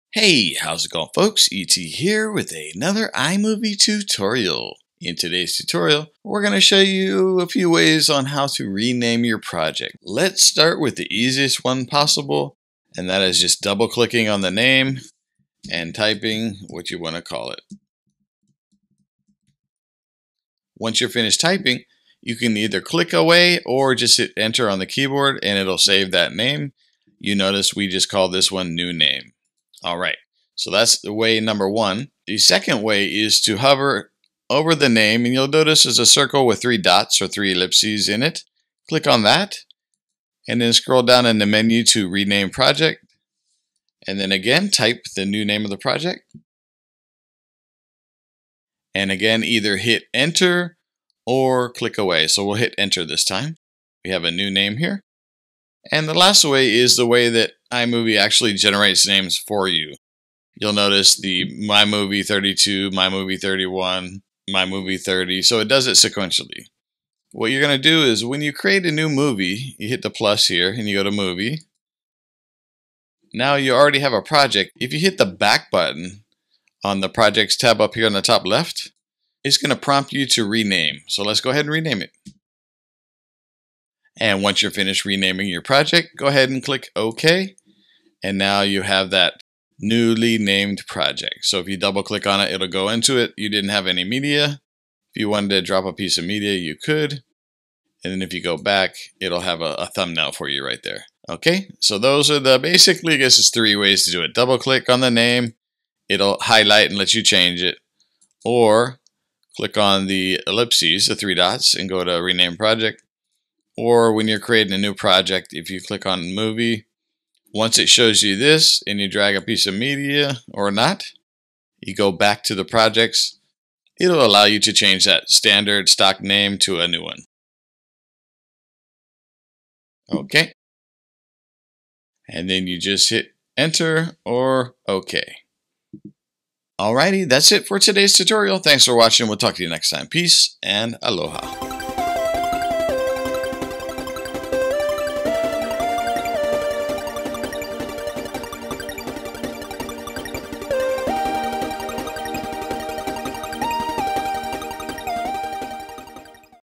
iMovie Tutorial - How to Rename Movie Projects
In this Apple iMovie Tutorial for MacOS, I show you 3 different ways to rename your movie video projects.

ET